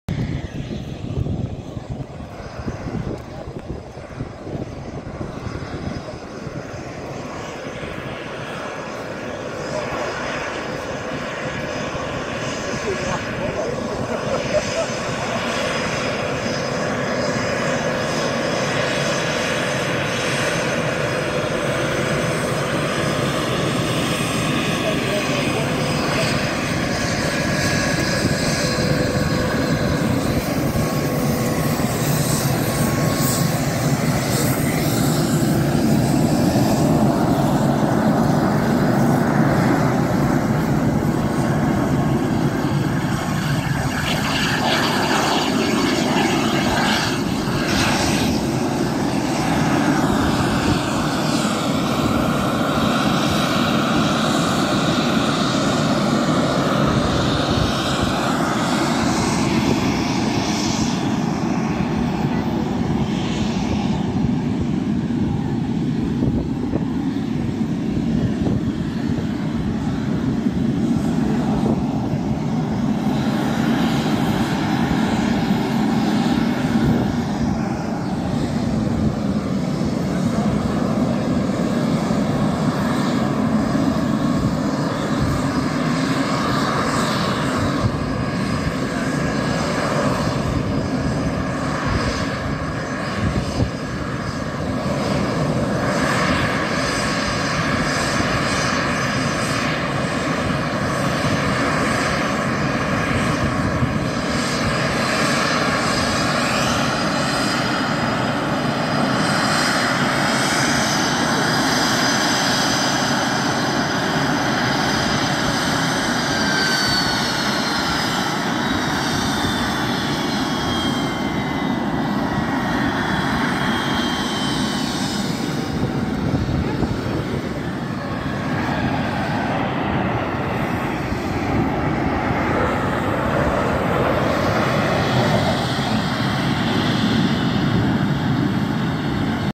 Swedish Jas-39 Gripen returning from air display.
The Royal International Air Tattoo, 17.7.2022.